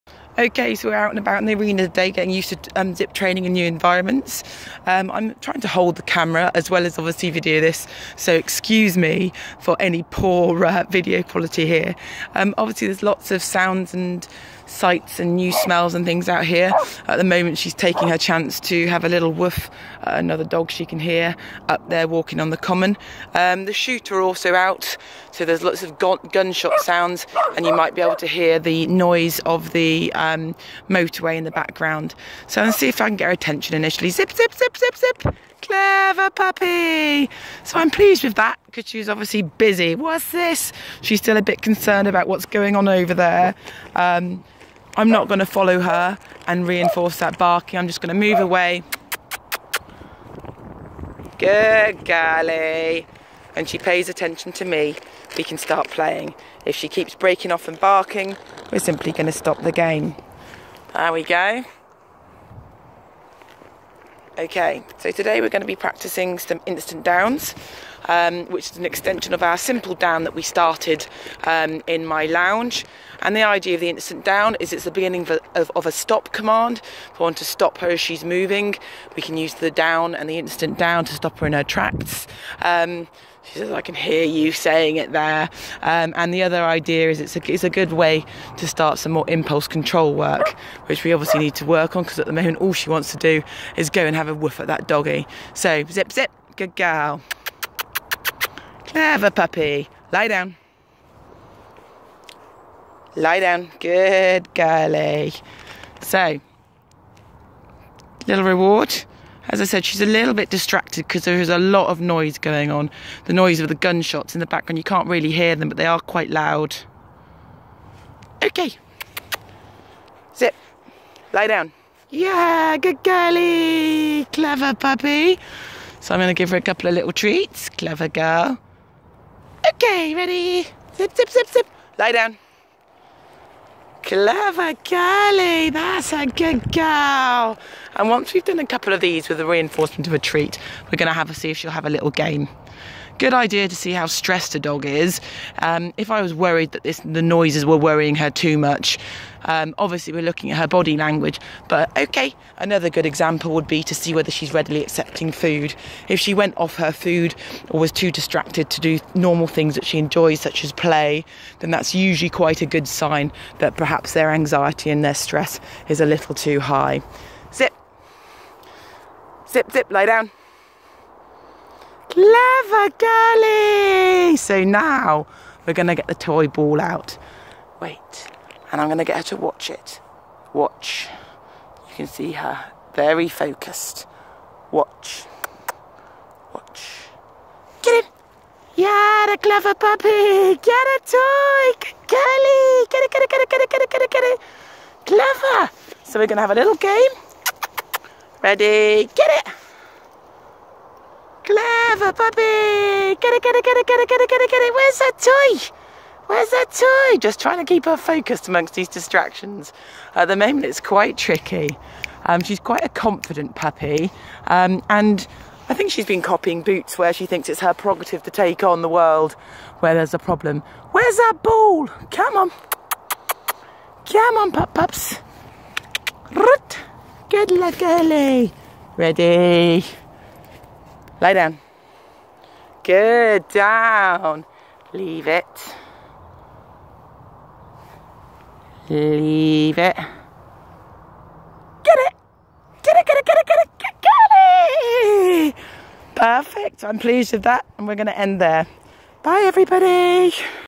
Sheepdog training. The Instant Down
Teaching a 12 week old border collie pup the Instant Down